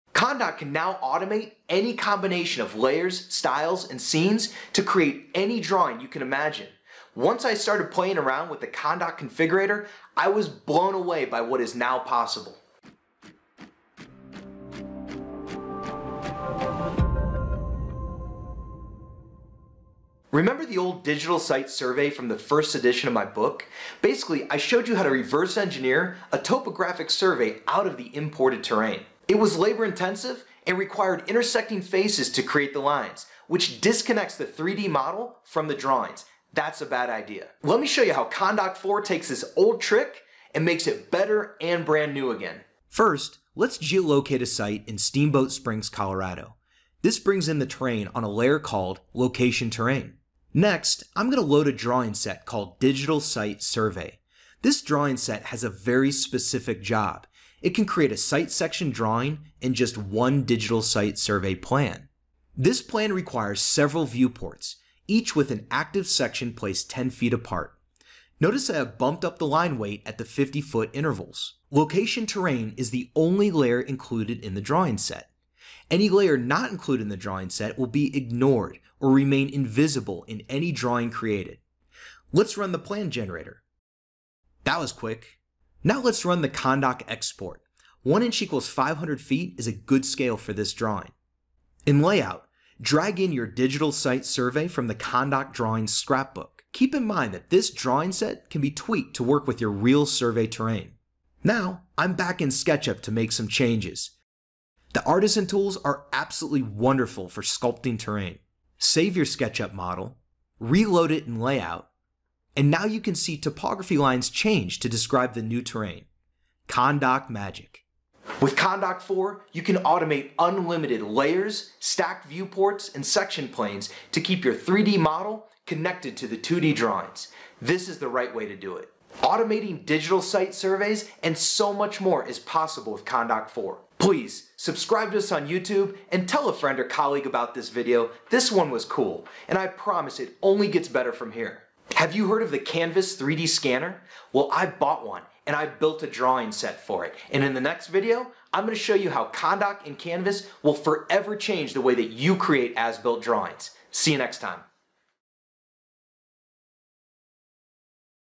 ConDoc v4 Teaser 4: Watch Closely, Or You Will Miss This Sketchup Magic Trick!
ConDoc version 4 has breathed new life into an old demo of a digital site survey. Using a custom drawing set, we're able to create an accurate, geo-located terrain map with only a few clicks. The best part is, the terrain file is linked to your drawings, so any changes you make to the terrain will update your topo map automatically! No more time lost manually adjusting all your section cuts. Let ConDoc do the heavy lifting!